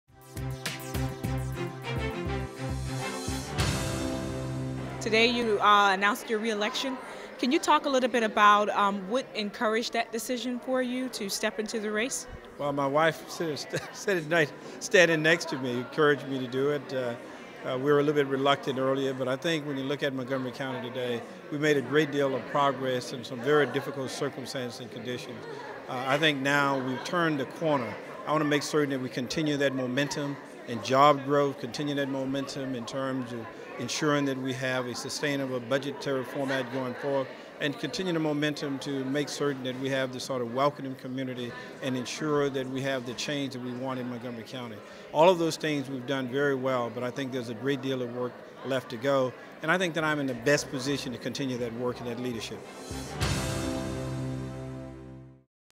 County Executive Ike Leggett On Why He Is Running For Re-Election a MyMCMedia Extra
The day Montgomery County Executive Ike Leggett announced he would run for a third term, MyMCMedia's Tamika Smith asked him why he decided to run for re-election in 2014.
June 13, 2013. Tamika Smith reporting.